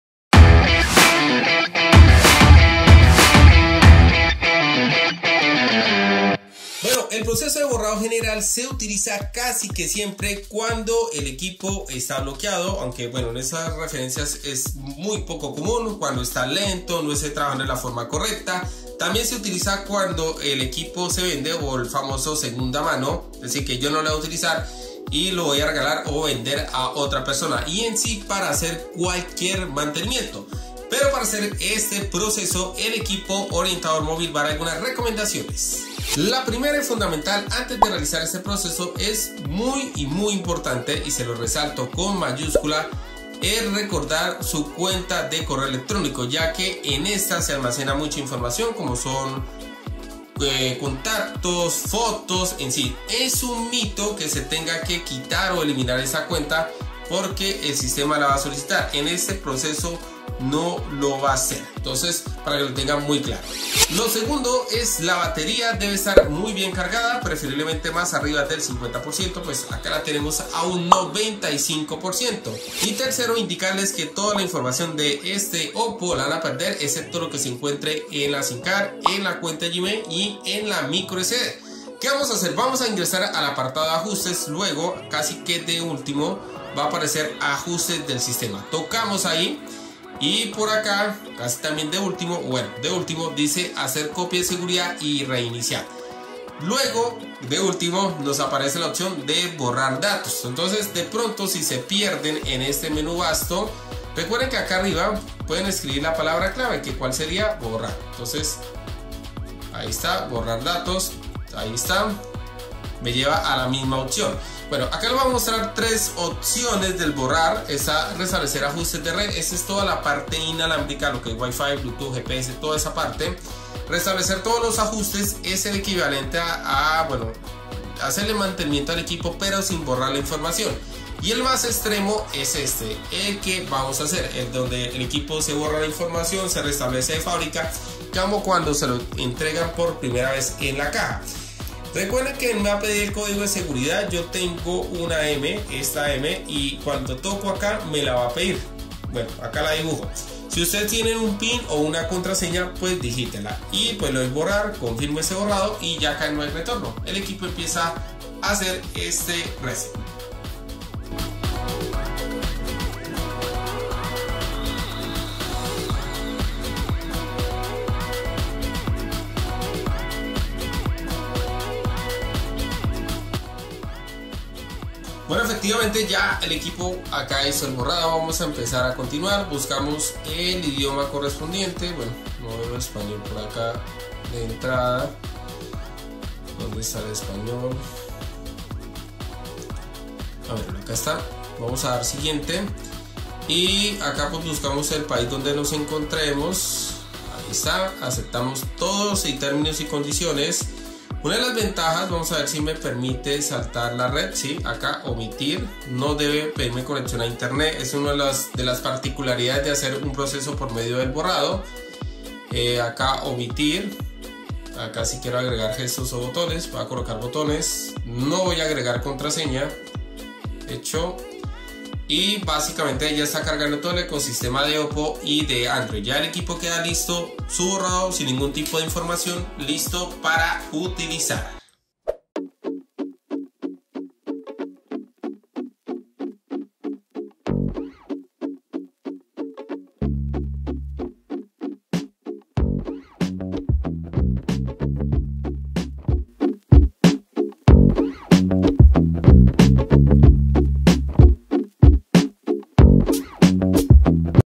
OPPO RENO 7 📲 Restaurar De Fabrica O Borrado General de tu MÓVIL📱
Aprende a hacer Hard Reset tu OPPO RENO 7    , de una manera muy fácil y rápido, quitando contraseñas, patrón o pin.

▶️ Aquí lo puedes adquirir ✔